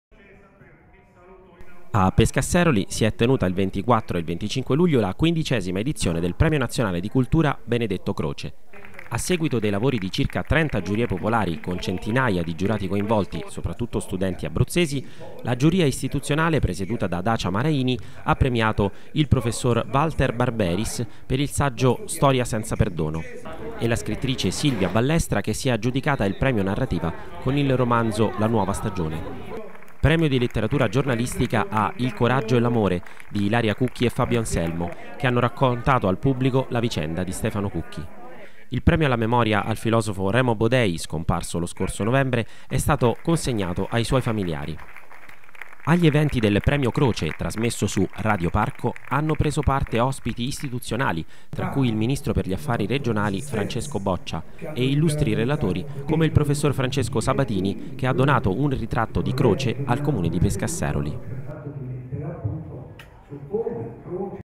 Pescasseroli. Premio Benedetto Croce
Si è tenuta, la scorsa settimana a Pescasseroli, la 15° edizione del Premio Nazionale di Cultura Benedetto Croce